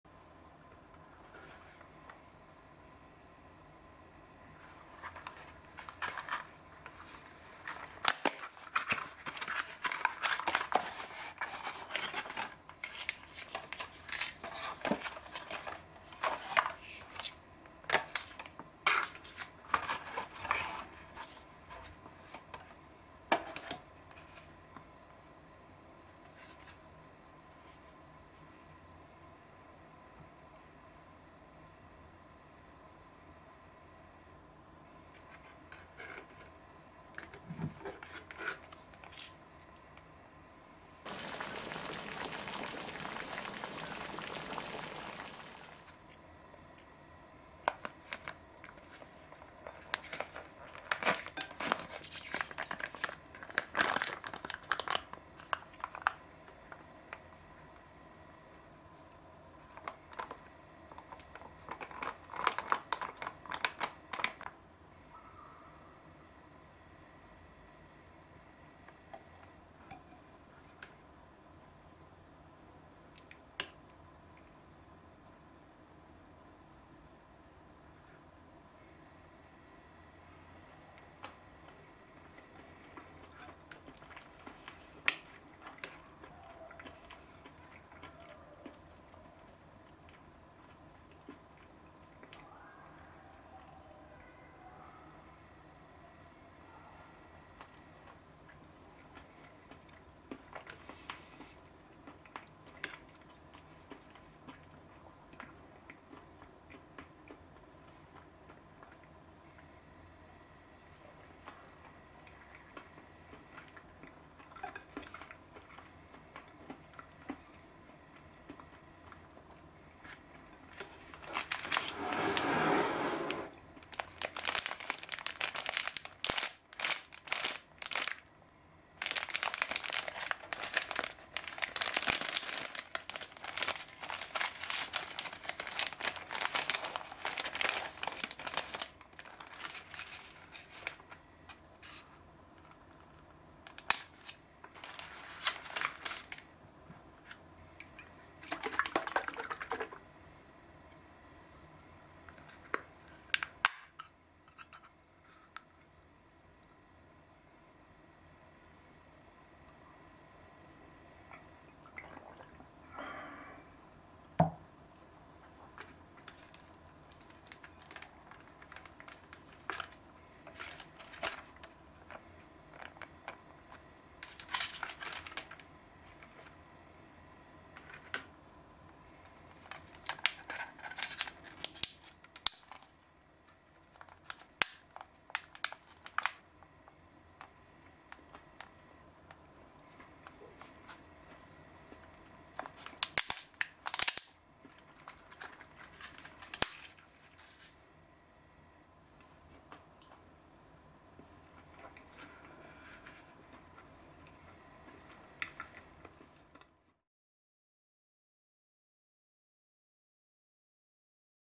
EVANGELION Retort Pouch Curry and Cheese Tart
EVANGELION retort pouch curry, cheese tart and canned coffee. Cheese tart is Familymart limited and yummy. The canned coffee is sweet milk coffee.